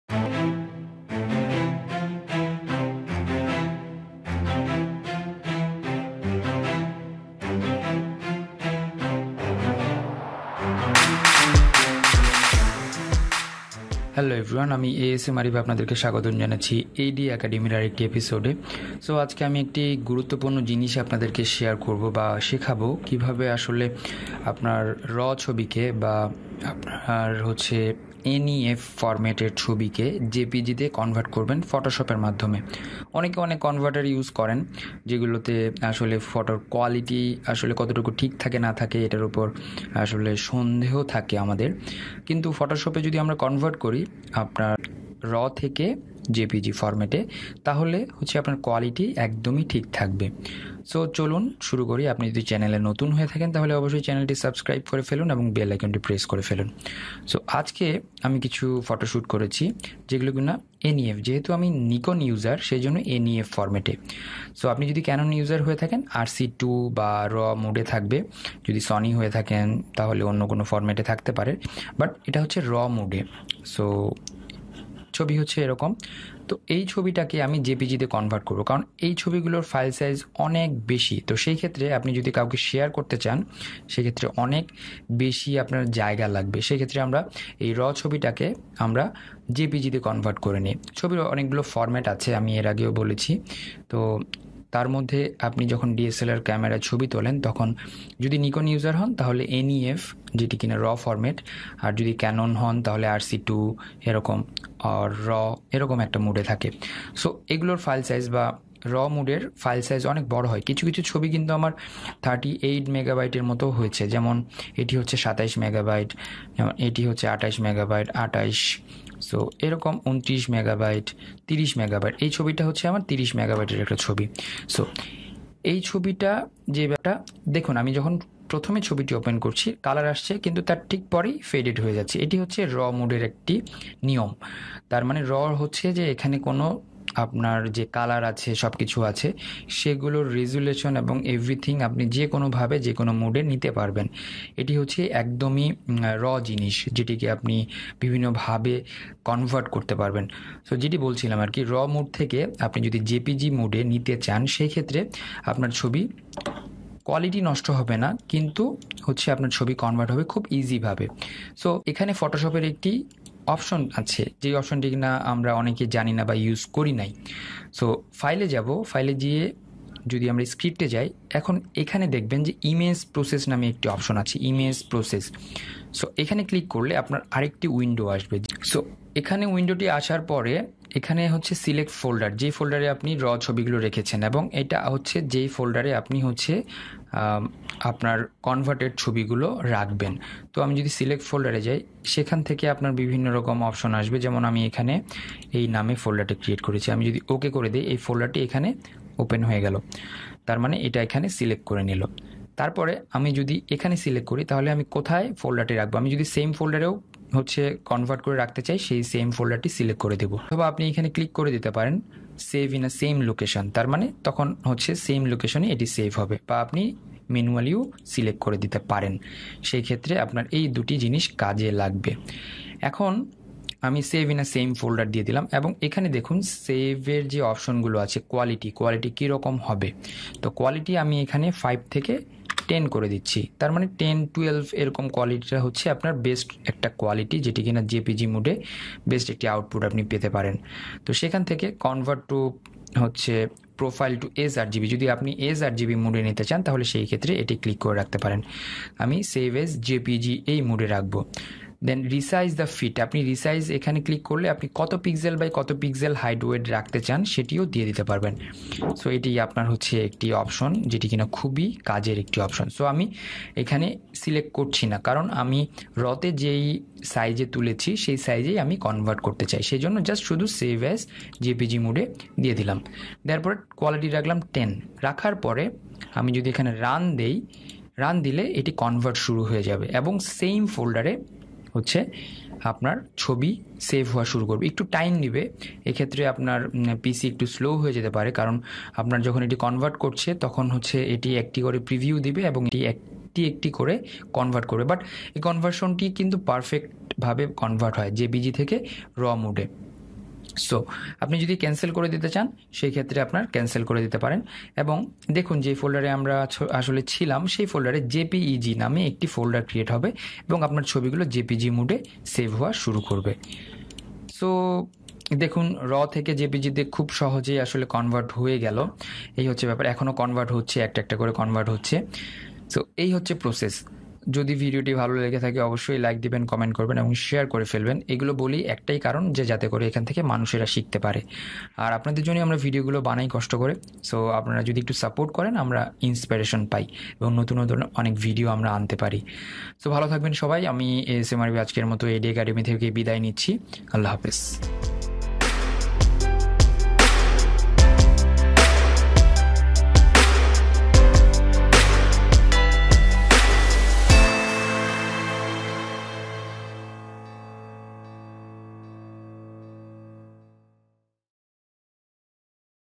RAW Files to JPEG in Adobe Photoshop (Batch Convert) | একসাথে ছবি কনভার্ট করুন
Here's my new video showing you how to easily Batch Convert Raw files to JPEG! Enjoy! RAW Files to JPEG in Adobe Photoshop (Batch Convert) | একসাথে ছবি কনভার্ট করুন, Raw image format, JPEG, Computer file, adobe photoshop, photoshop, jpeg